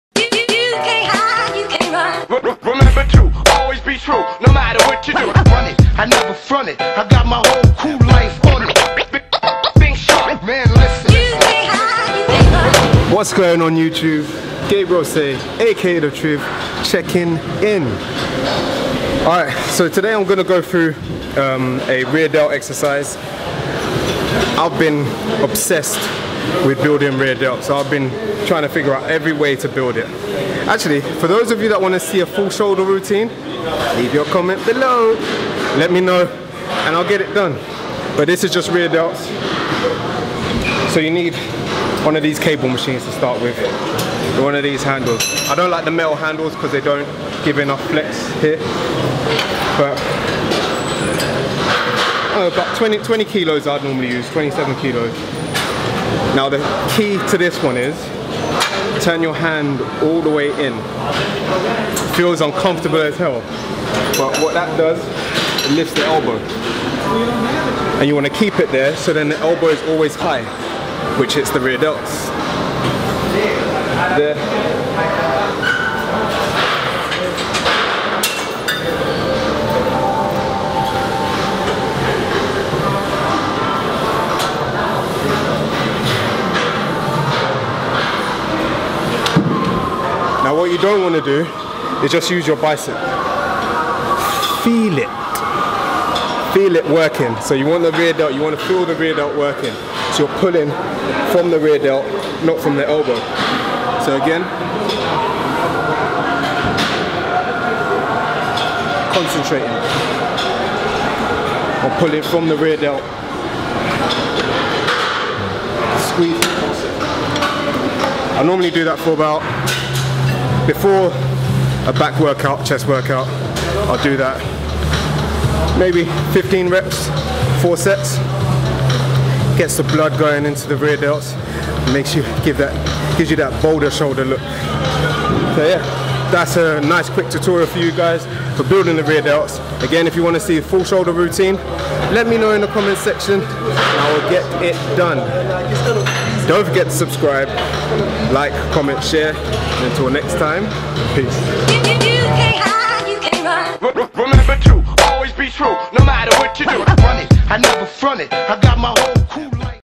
Rear Delt Building Exercise | Boulder Shoudler Series
Hey Guys, here is another exercise to add to my OBSESSION for rear delt exercises.. haha..